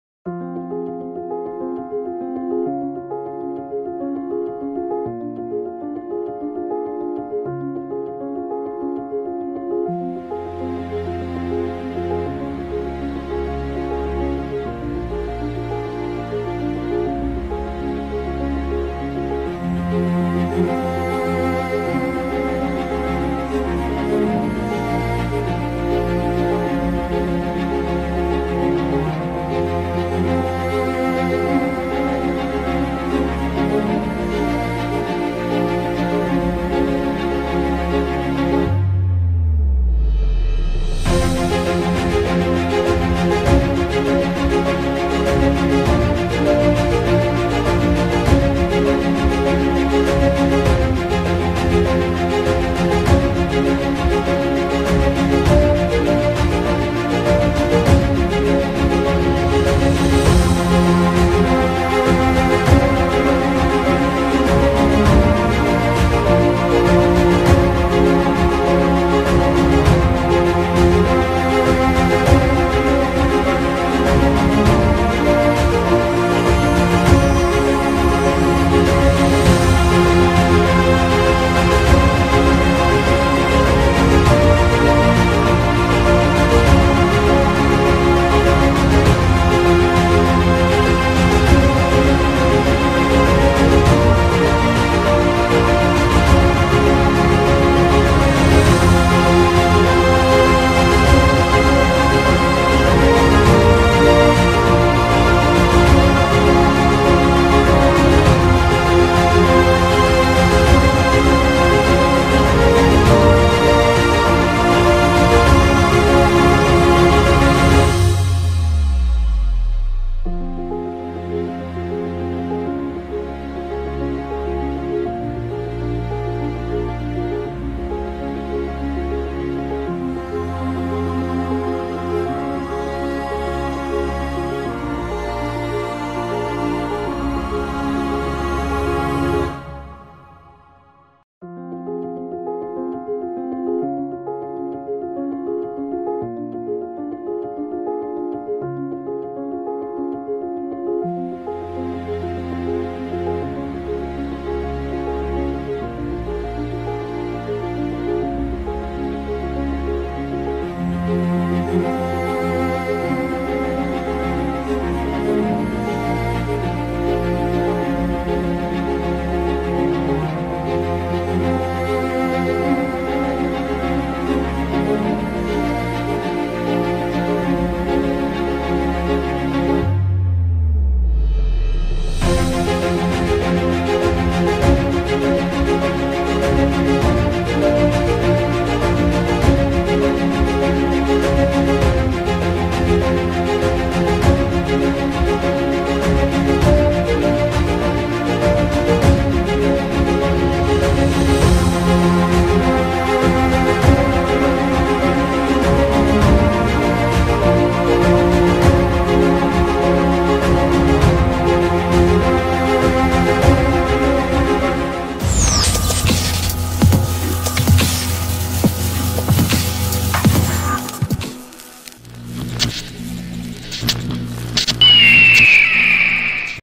BJK - KEVIN PRINCE BOATENG POSTER DESIGN (photoshop)
BJK - yeni transferi BOATENG in poster tasarım videosu sizlerle... Umarım beğenirsiniz :)